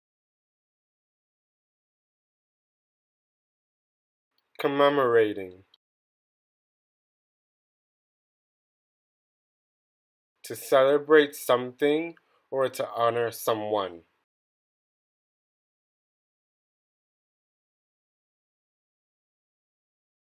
The Nature of Happiness - Commemorating
This is a vocabulary video with an ASL translation of the definition. This video is part of the vocabulary set for the “Nature of Happiness” lesson plan. English captions and English voiceover are available.